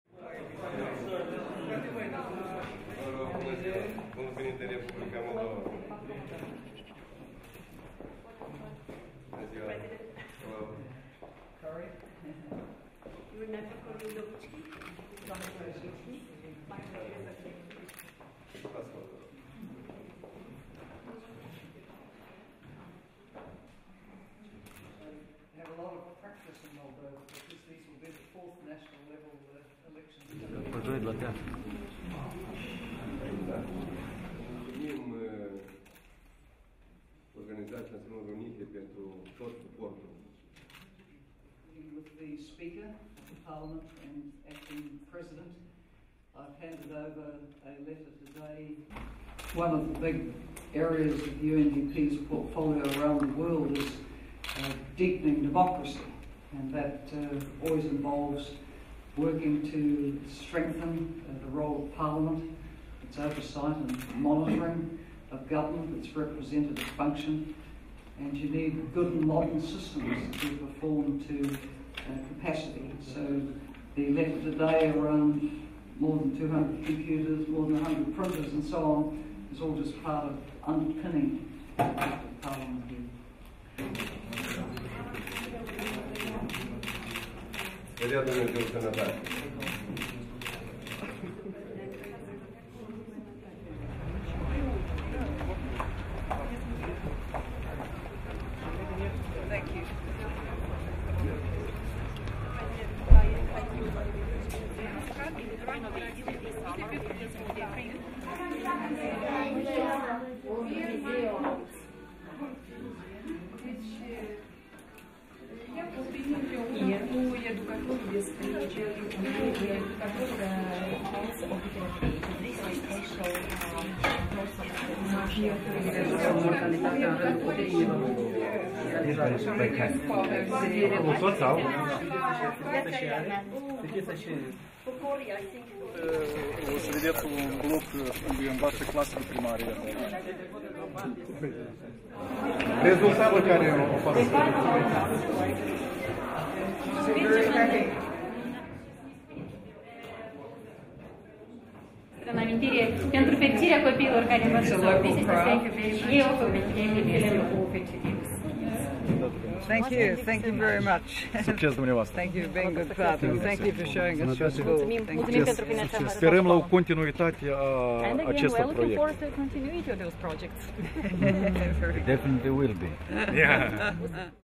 Helen Clark in Moldova - day two of official visit
10 September 2010
UNDP Administrator Helen Clark meets Acting President Mihai Ghimpu and visits UNDP projects in several communities in Moldova.